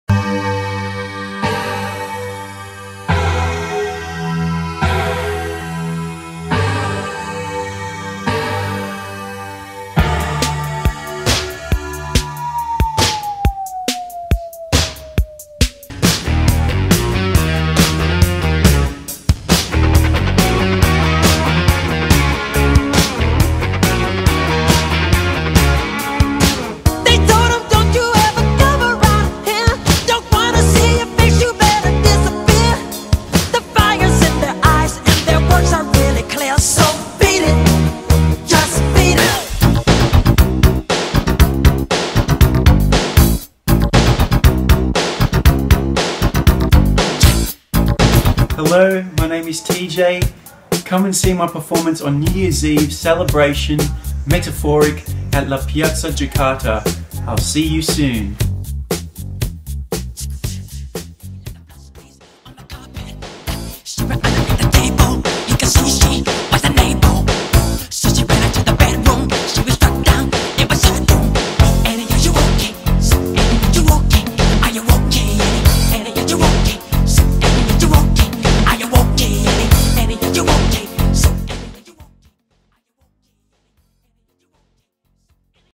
METAMORPHIC - New Year's Eve Celebration at La Piazza
Celebrate New Year`s Eve in Evolution of Light

"METAMORPHIC"
31 December 2014 | start from 7 PM  | at La Piazza

Guest Star:
* Duo Percussion
* The Dance Company
* G.A.C
* DJ Yellow

Special performance by TJ Capolla - Australia #1 Michael Jackson Impersonator

Entertainments:
Band & DJ Performance | LED Dancer | Flashmob | Game Booth | Extreme Lasermen X | Duo Mirror Dancers

Early Bird Ticket (until 21 Dec 2014) IDR. 100.000
Show Day Ticket IDR. 125.000

New Year Dinner Package:
Starting IDR 325.000++

RSVP:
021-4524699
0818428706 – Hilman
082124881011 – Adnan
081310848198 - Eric

“Wear your glow in the dark attribute and don’t stop glowing!”